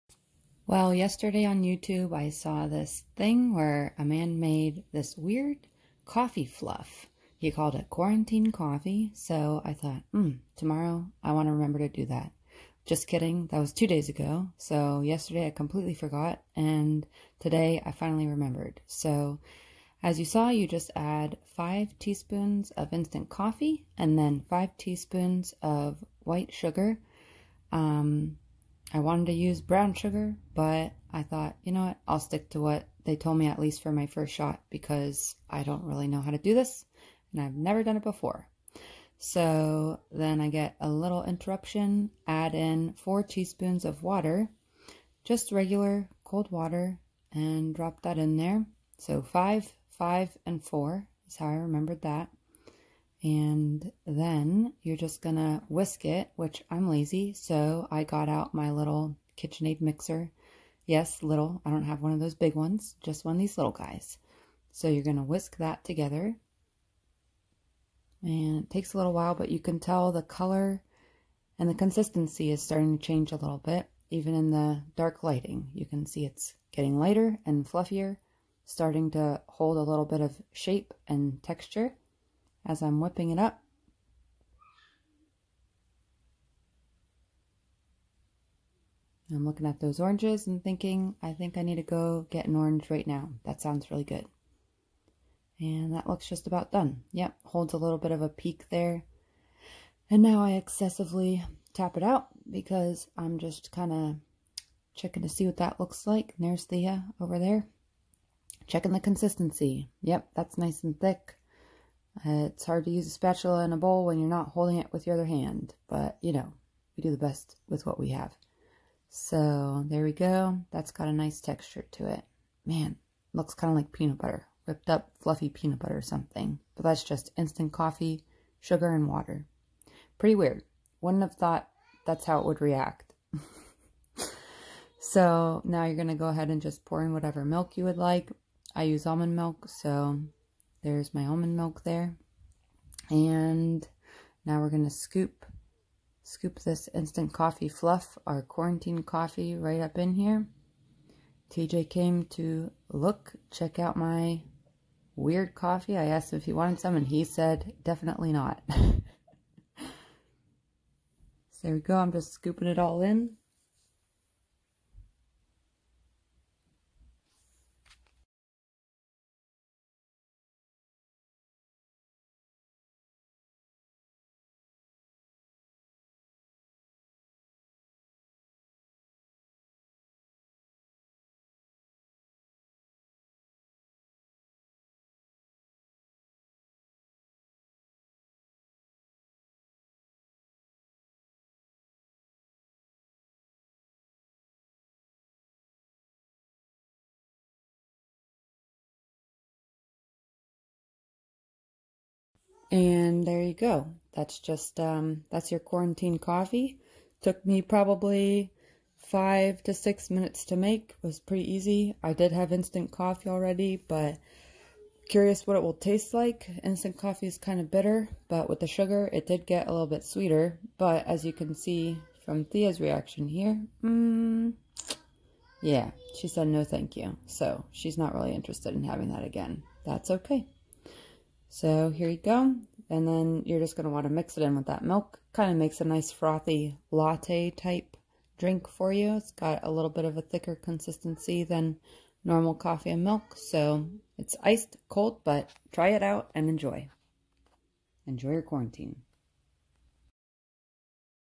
Quarantine Coffee
Thought I would take advantage of our slower moving days to try out something I saw here on YouTube... Quarantine coffee!

5 tsp instant coffee
5 tsp sugar
4 tsp water

Whisk
Add milk